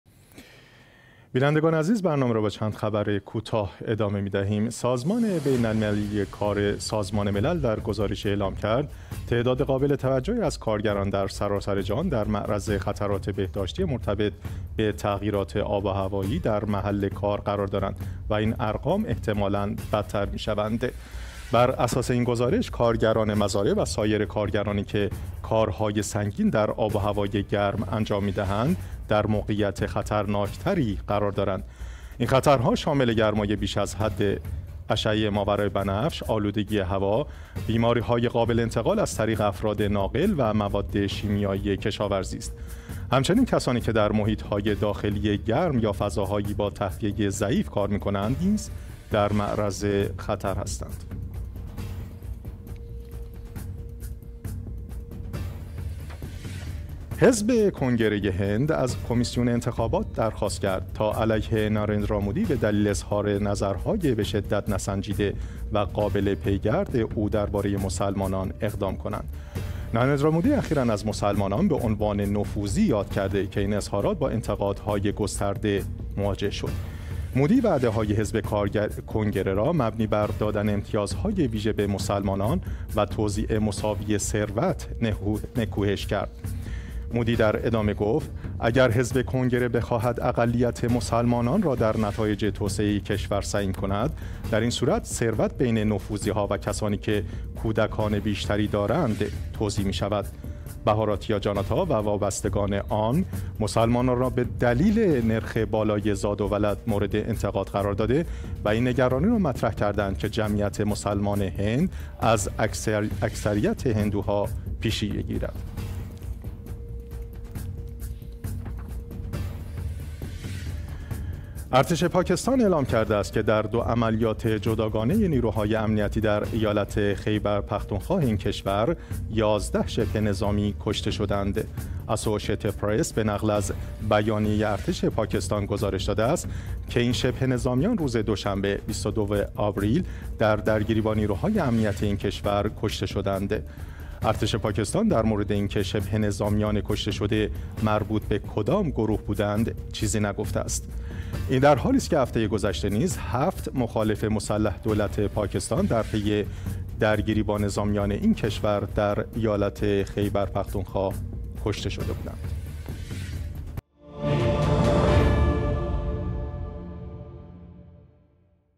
چند خبر کوتاه : سه شنبه 4 اردیبهشت ماه 1403
هشدار سازمان ملل در مورد وضعیت خطرناک بهداشتی کارگران

حزب کنگره هند به دنبال اقدام قانونی علیه اظهارات مودی در مورد مسلمانان

دست‌کم ۱۱ شبه‌نظامی در منطقه شمال‌غربی پاکستان هم مرز با افغانستان کشته شدند